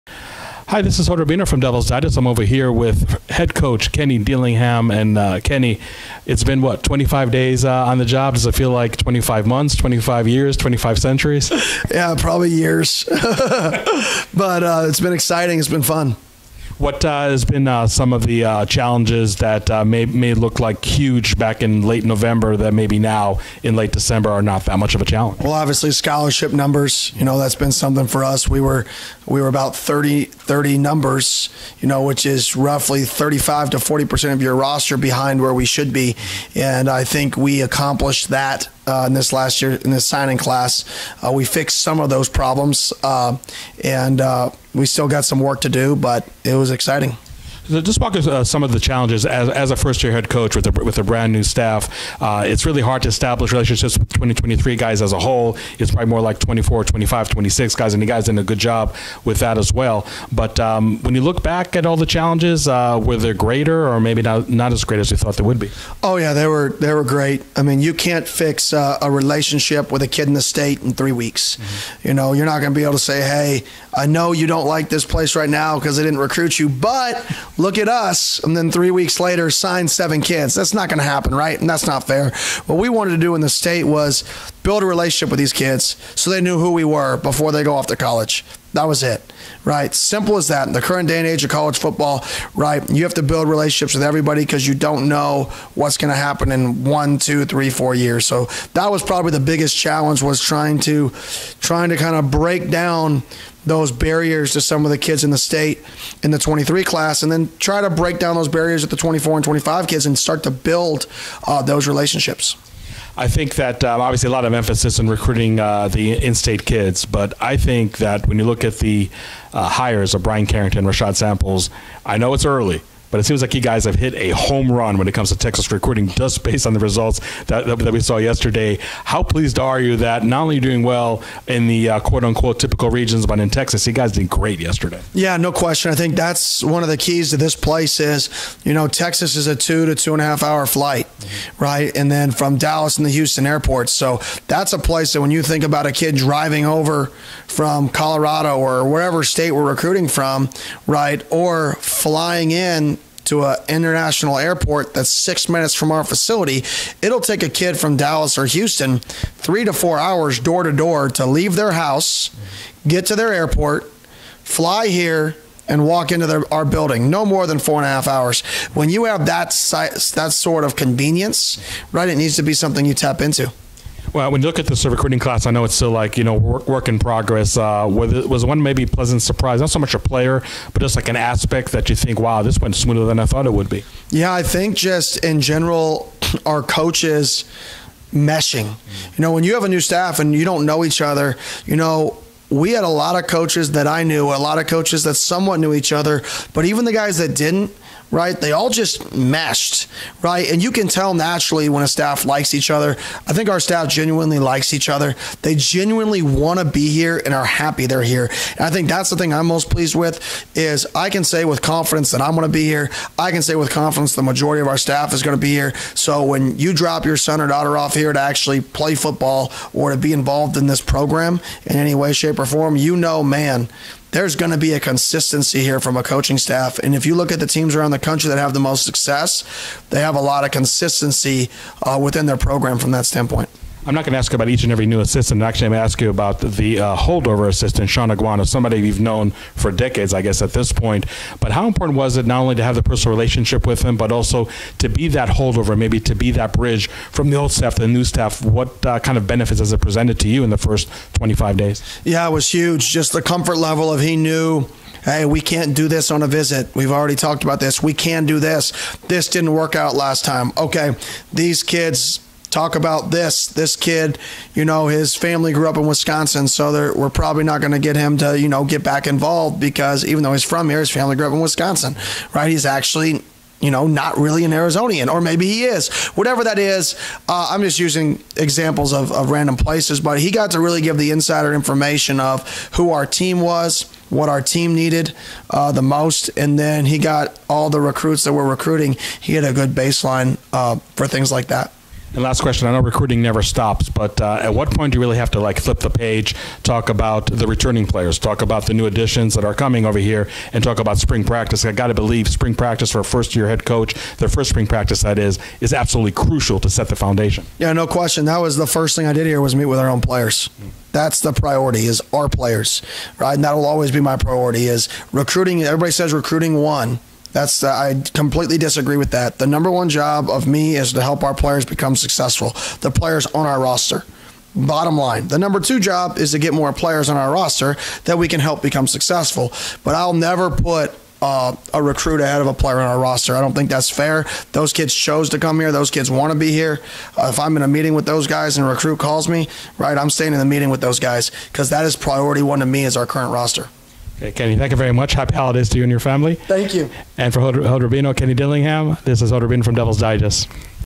DevilsDigest TV: Kenny Dillingham talks about the first 25 days on the job, recruiting philosophy
Our one-on-one interview with ASU's head coach (Alex Gould/The Republic / USA TODAY NETWORK)